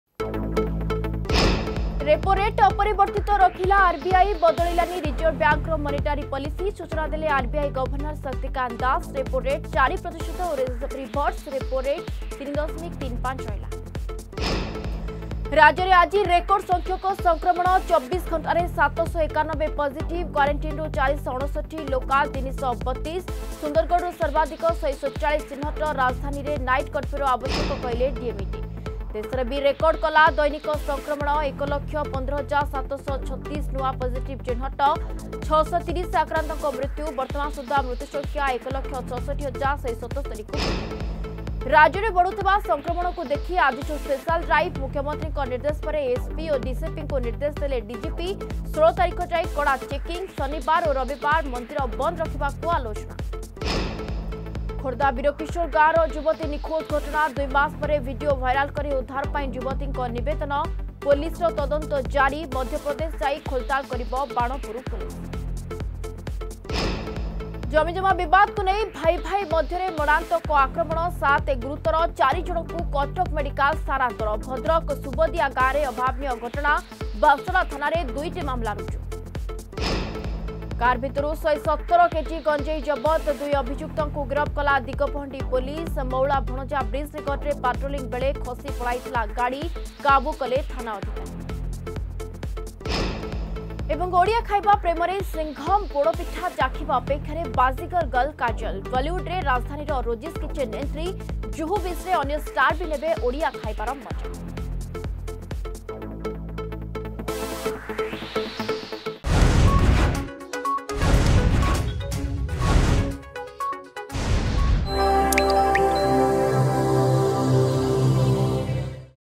Headlines 12pm
ରେପୋ ରେଟ ଅପରିବର୍ତ୍ତିତ ରଖିଲା ଆରବିଆଇ...
ରାଜ୍ୟରେ ଆଜି ରେକର୍ଡ ସଂଖ୍ୟକ ସଂକ୍ରମଣ …
ଦେଶରେ ବି ରେକର୍ଡ କଲା ଦୈନିକ ସଂକ୍ରମଣ...
ରାଜ୍ୟରେ ବଢ଼ୁଥିବା ସଂକ୍ରମଣକୁ ଦେଖି ଆଜିଠୁ ସ୍ପେଶାଲ ଡ୍ରାଇଭ...
ଖୋର୍ଦ୍ଧା ବୀରକିଶୋର ଗାଁର ଯୁବତୀ ନିଖୋଜ ଘଟଣା...
ଜମିଜମା ବିବାଦକୁ ନେଇ ଭାଇ ଭାଇ ମଧ୍ୟରେ ମରଣାତ୍ମକ ଅକ୍ରମଣ...
କାର ଭିତରୁ 117 କେଜି ଗଞ୍ଜେଇ ଜବତ...
ଓଡ଼ିଆ ଖାଇବା ପ୍ରେମରେ ସଂଘମ୍...